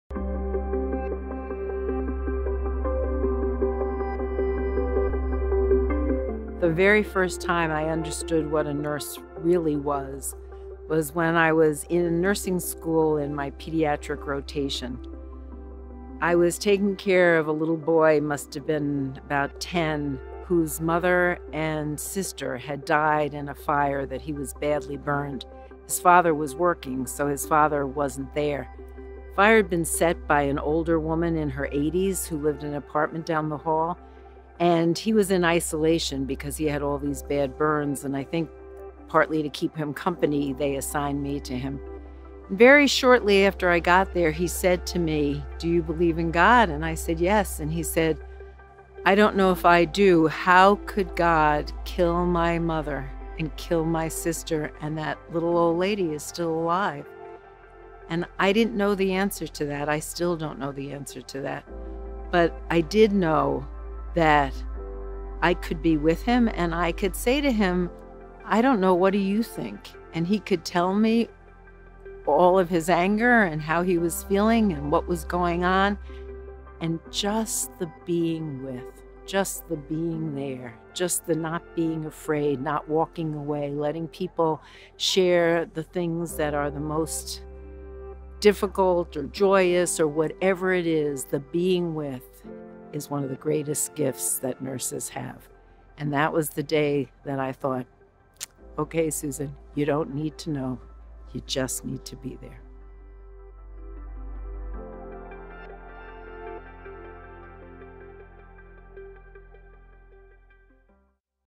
Boston College Celebrates Year of the Nurse | Dean Susan Gennaro
Boston College Connell School of Nursing Dean Susan Gennaro recounts the story she realized what a "nurse really is."